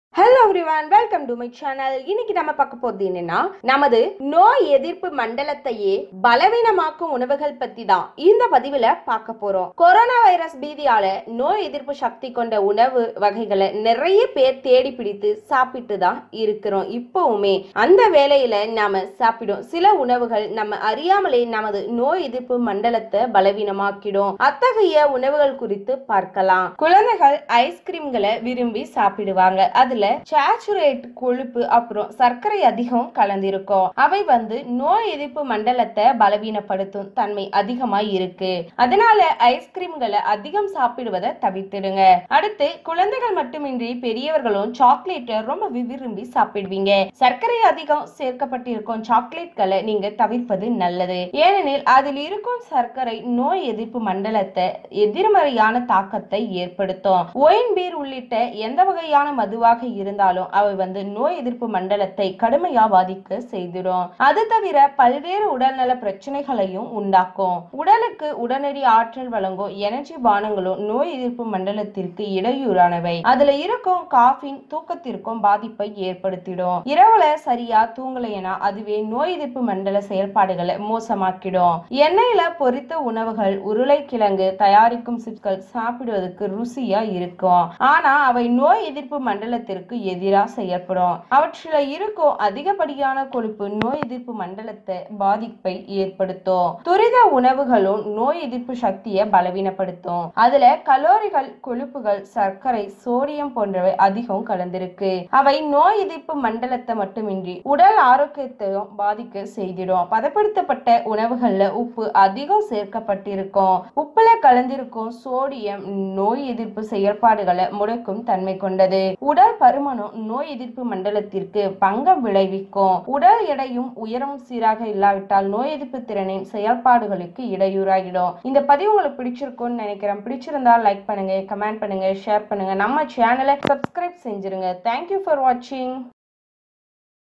நமது நோய் எதிர்ப்பு மண்டலத்தை பலவீனமாக்கும் உணவுகள்
கொரோனா வைரஸ் பீதியால் நோய் எதிர்ப்பு சக்தி கொண்ட உணவு வகைகளை நிறைய பேர் தேடிப்பிடித்து சாப்பிட்டுக்கொண்டிருக்கிறார்கள். அதேவேளையில் நாம் சாப்பிடும் சில உணவுகள் நம்மை அறியாமலேயே நமது நோய் எதிர்ப்பு மண்டலத்தை பலவீனமாக்கக்கூடியவை. அத்தகைய உணவுகள் குறித்து பார்ப்போம்.

குழந்தைகள் ஐஸ்கிரீம்களை விரும்பி சாப்பிடுவார்கள். அதில் சாச்சுரேட்டட் கொழுப்பு(saturated fat ) மற்றும் சர்க்கரை அதிகம் கலந்திருக்கும். அவை நோய் எதிர்ப்பு மண்டலத்தை பலவீனப்படுத்தும் தன்மை கொண்டவை. அதனால் ஐஸ்கிரீம்களை அதிகம் சாப்பிடுவதை தவிர்க்க வேண்டும்.

குழந்தைகள் மட்டுமின்றி பெரியவர்களும் சாக்லேட்டை விரும்பி சாப்பிடுகிறார்கள். சர்க்கரை அதிகம் சேர்க்கப்பட்டிருக்கும் சாக்லேட்டுகளை தவிர்ப்பது நல்லது. ஏனெனில் அதிலிருக்கும் சர்க்கரை நோயெதிர்ப்பு மண்டலத்தில் எதிர்மறையான தாக்கத்தை ஏற்படுத்தக்கூடியவை.

ஒயின், பீர் உள்பட எந்தவகையான மதுவாக இருந்தாலும் அவை நோய் எதிர்ப்பு மண்டலத்தை கடுமையாக பாதிக்கச் செய்துவிடும். அதுதவிர பல்வேறு உடல்நல பிரச்சினைகளையும் உண்டாக்கும்.

உடலுக்கு உடனடி ஆற்றல் வழங்கும் எனர்ஜி பானங்களும் நோய் எதிர்ப்பு மண்டலத்திற்கு இடையூறானவை. அதில் இருக்கும் காபின் தூக்கத்திற்கும் பாதிப்பை ஏற்படுத்தக்கூடும். இரவில் சரியாக தூங்காவிட்டால் அதுவே நோய் எதிர்ப்பு மண்டல செயல்பாடுகளை மோசமாக்கி விடும்.

எண்ணெய்யில் பொரித்த உணவுகள், உருளைக்கிழங்கில் தயாராகும் சிப்ஸ்கள் சாப்பிடுவதற்கு ருசியாக இருக்கலாம். ஆனால் அவை நோய் எதிர்ப்பு மண்டலத்திற்கு எதிரானவை. அவற்றில் இருக்கும் அதிகப்படியான கொழுப்புகள் நோய் எதிர்ப்பு மண்டலத்திற்கு பாதிப்பை ஏற்படுத்தும்.

துரித உணவுகளும் நோய் எதிர்ப்பு சக்தியை பலவீனப்படுத்தக்கூடியவை. அதில் கலோரிகள், கொழுப்புகள், சர்க்கரை, சோடியம் போன்றவை அதிகம் கலந்திருக்கும். அவை நோய் எதிர்ப்பு மண்டலத்தை மட்டுமின்றி உடல் ஆரோக்கியத்தையும் பாதிக்க வைத்துவிடும்.

பதப்படுத்தப்பட்ட உணவுகளில் உப்பு அதிகம் சேர்க்கப்பட்டிருக்கும். உப்பில் கலந்திருக்கும் சோடியம் நோய் எதிர்ப்பு செயல்பாடுகளை முடக்கும் தன்மை கொண்டவை.

உடல் பருமனும் நோய் எதிர்ப்பு மண்டலத்திற்கு பங்கம் விளைவிக்கும். உடல் எடையும், உயரமும் சீராக இல்லாவிட்டால் நோய் எதிர்ப்பு திறனின் செயல்பாடுகளுக்கு இடையூறாகிவிடும்.கொரோனா வைரஸ் பீதியால் நோய் எதிர்ப்பு சக்தி கொண்ட உணவு வகைகளை நிறைய பேர் தேடிப்பிடித்து சாப்பிட்டுக்கொண்டிருக்கிறார்கள். அதேவேளையில் நாம் சாப்பிடும் சில உணவுகள் நம்மை அறியாமலேயே நமது நோய் எதிர்ப்பு மண்டலத்தை பலவீனமாக்கக்கூடியவை. அத்தகைய உணவுகள் குறித்து பார்ப்போம்.

உடல் பருமனும் நோய் எதிர்ப்பு மண்டலத்திற்கு பங்கம் விளைவிக்கும். உடல் எடையும், உயரமும் சீராக இல்லாவிட்டால் நோய் எதிர்ப்பு திறனின் செயல்பாடுகளுக்கு இடையூறாகிவிடும்.